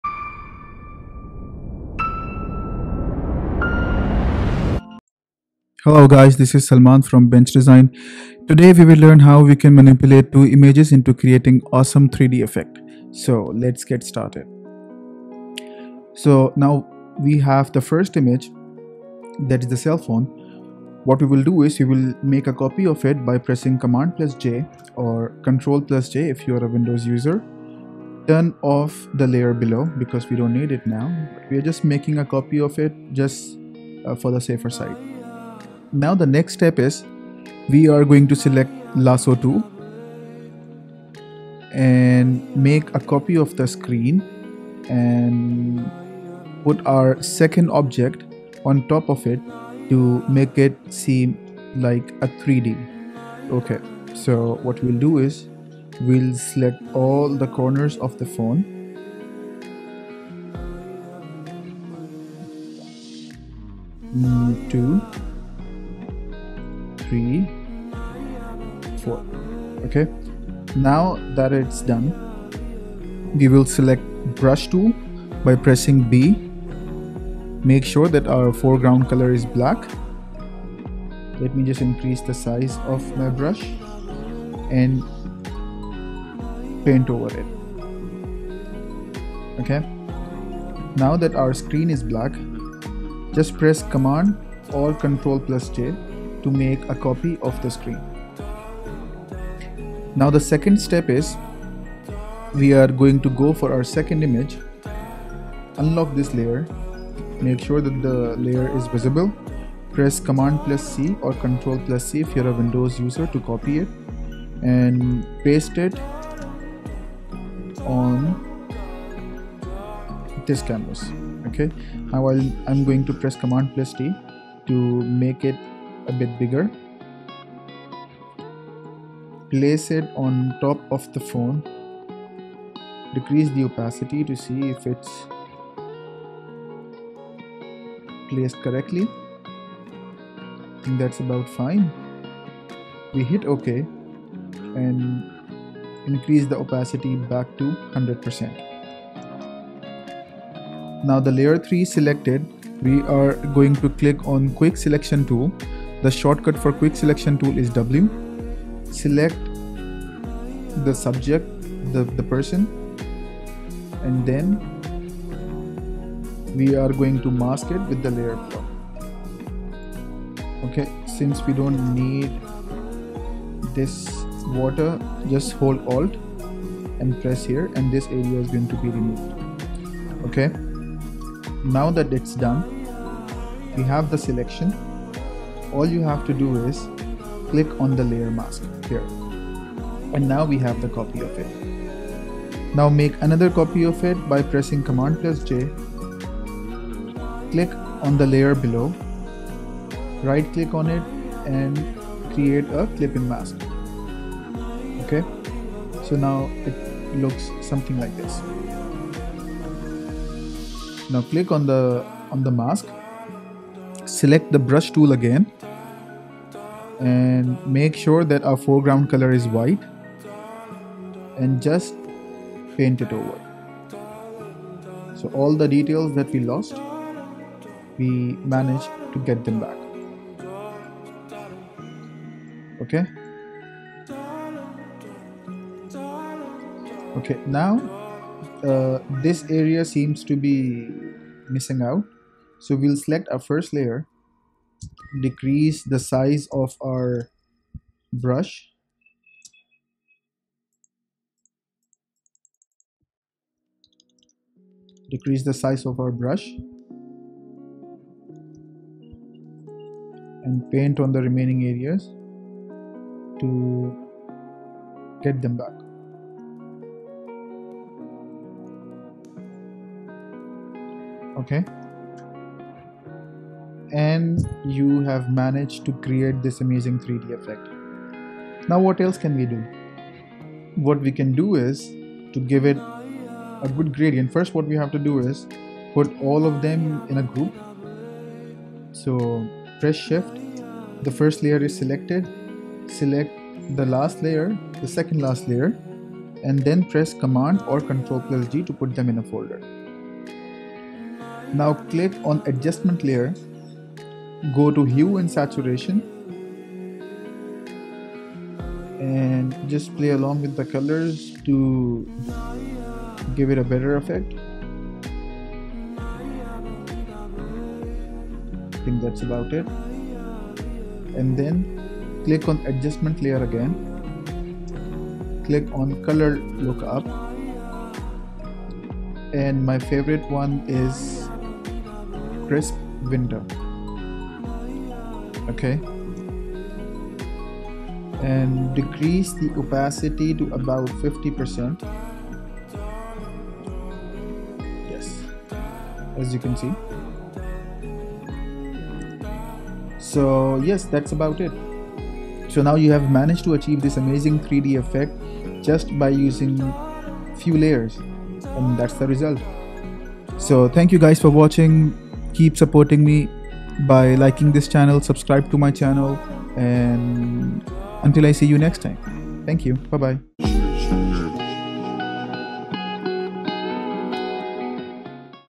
3D Photo Manipulation | Adobe Photoshop Tutorial CC 2020
In this tutorial, I will show you how you can easily manipulate two images to create this beautiful 3D effect using adobe photoshop.